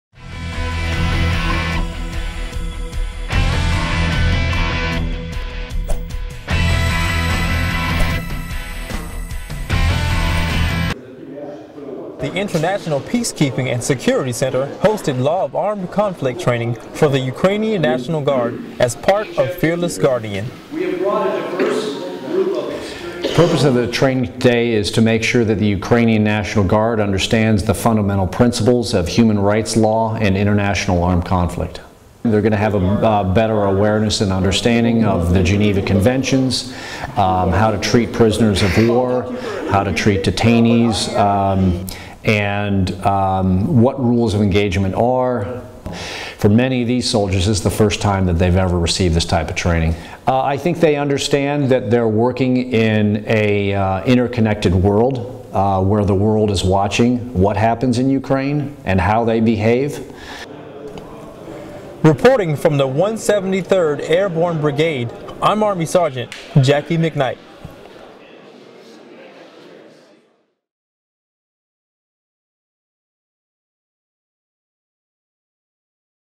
Law of Armed Conflict
The International Peacekeeping and Security Center hosted Law of Armed Conflict training for the Ukrainian National Guard.  Service members from the Defense Institute of International Legal Studies came to Yavoriv, Ukraine to deliver legal guidance